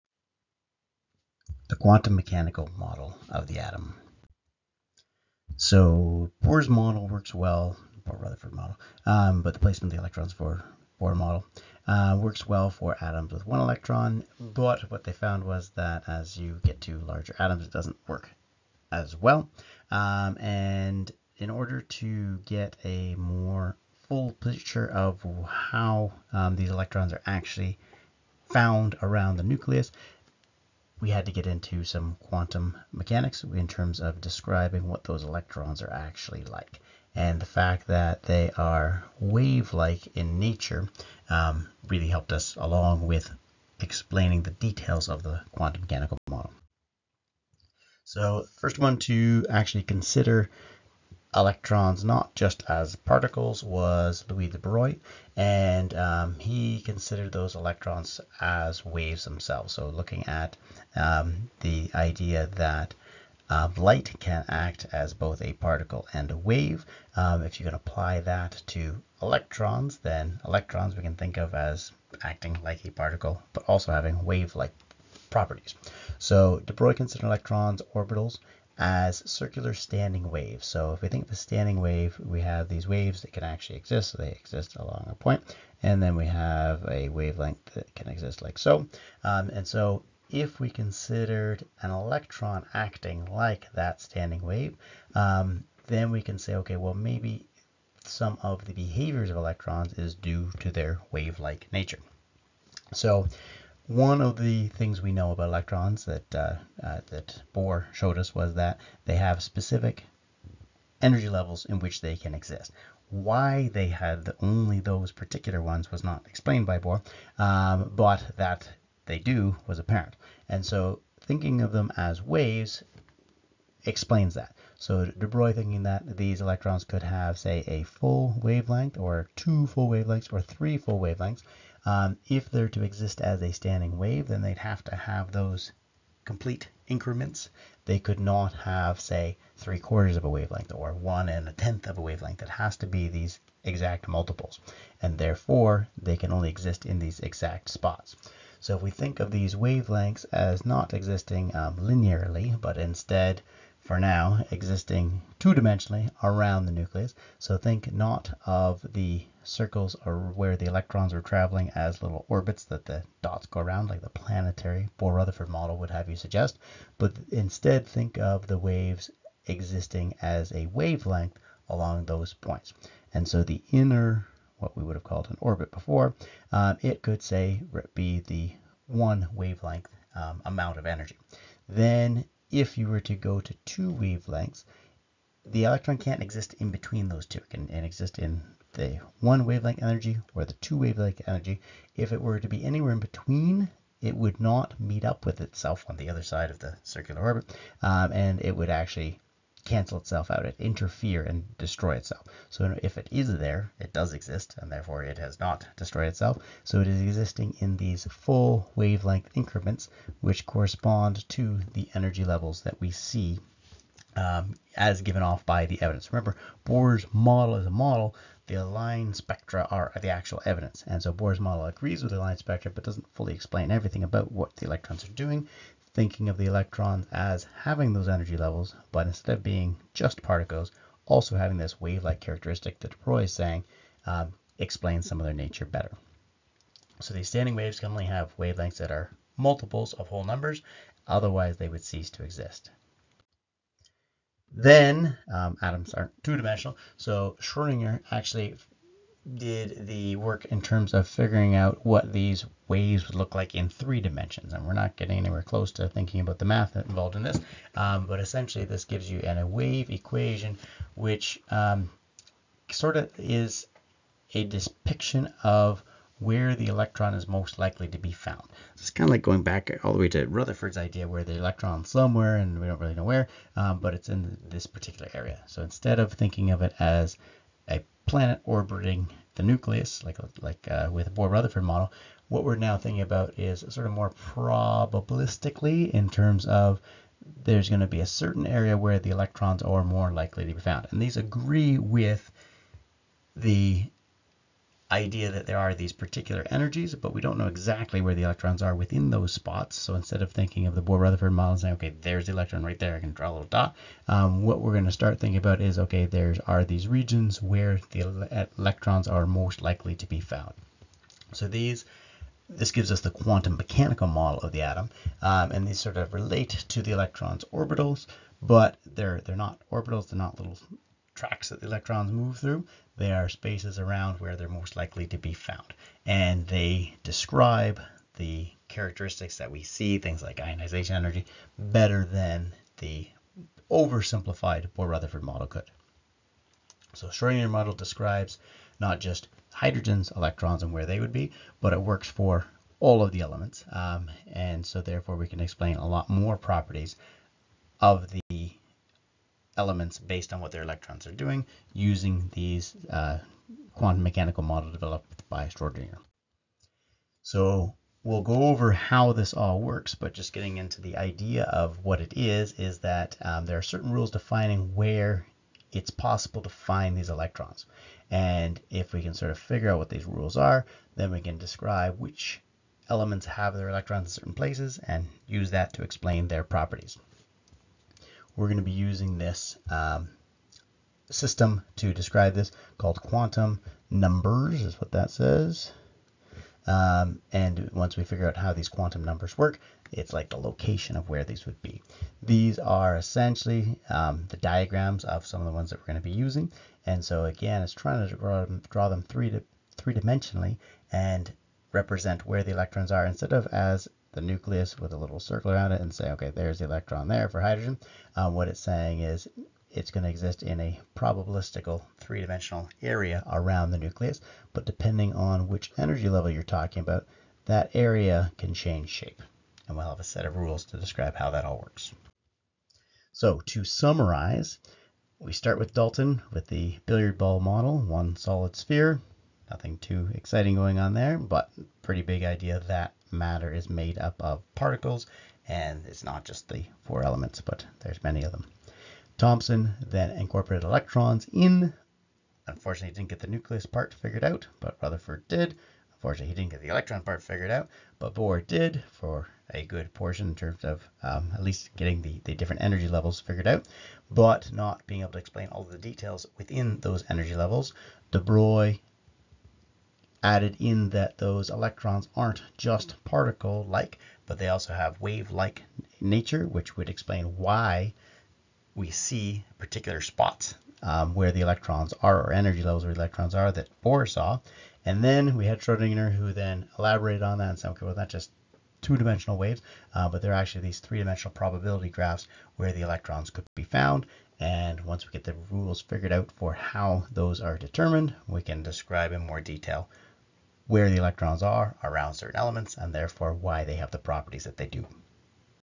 Quantum Mechanical Model of the Atom
A brief explanation of the quantum theory necessary to start using the quantum mechanical model of the atom.